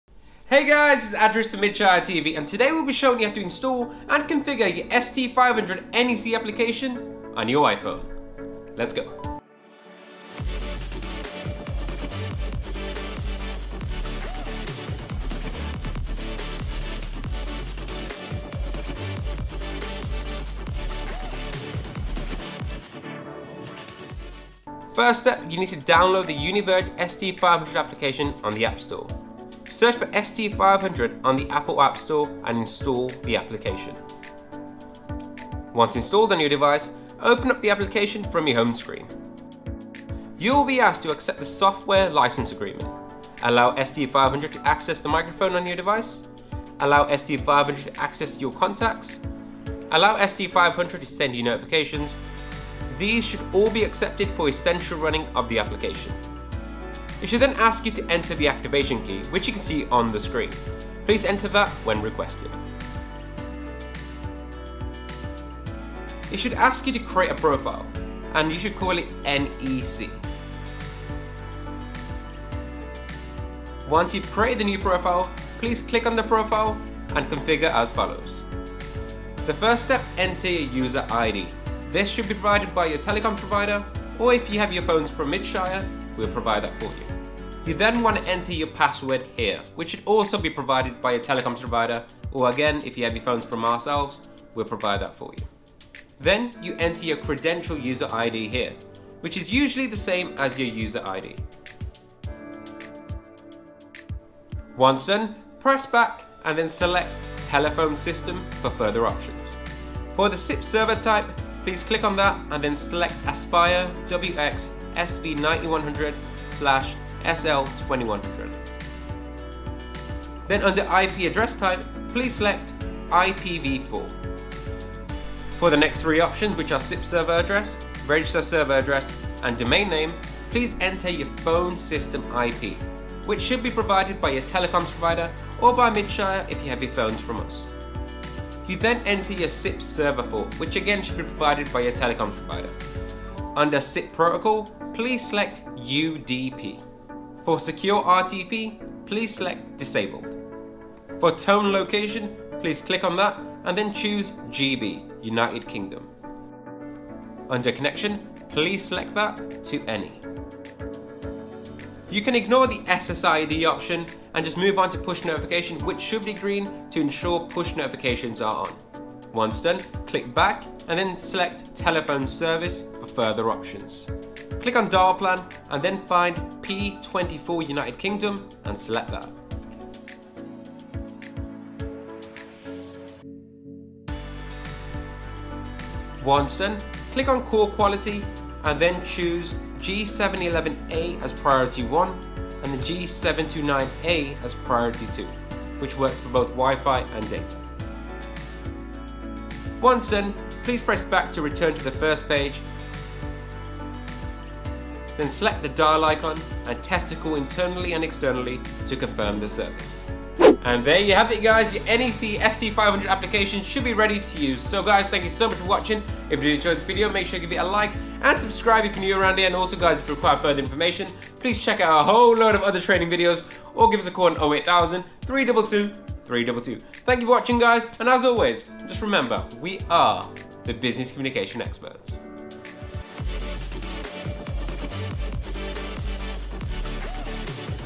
How to Install and Configure NEC ST500 App for iOS
Hi there, today we will be demonstrating how to install and configure the ST500 NEC Application for iOS.